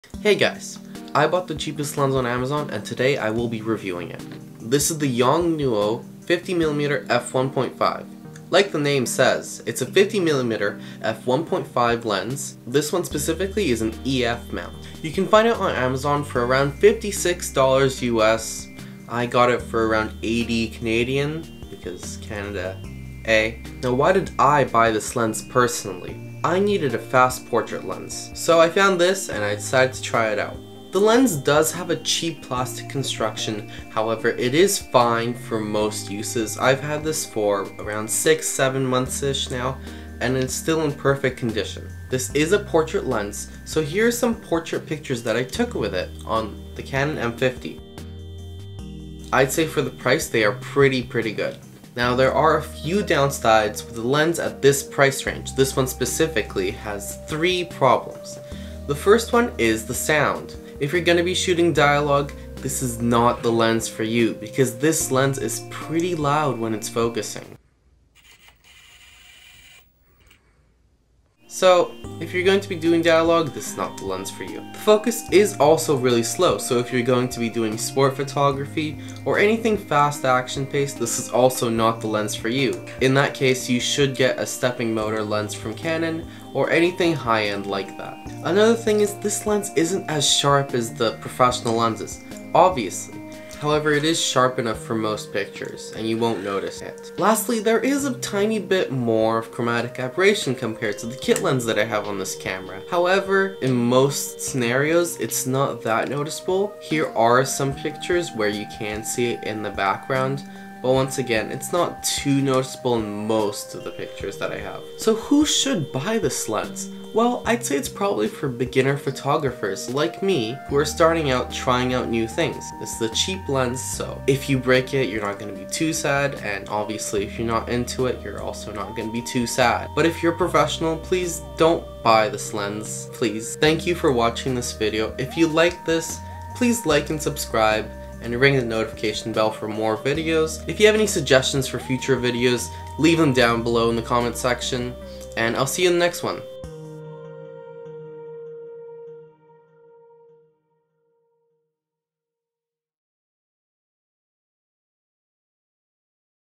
CHEAPEST lens on AMAZON | Is It Worth It?
Hey guys i bought the cheapest lens on amazon lol. it cost like 60 bucks USD. here is a review of it.

Timestamps:
00:00 Intro
00:23 Why did I buy it?
00:42 Pictures
00:52 Focusing speed&sound
01:28 Sharpness
01:36 Chromatic Aberration
01:54 Who should buy this lens?
02:12 Outro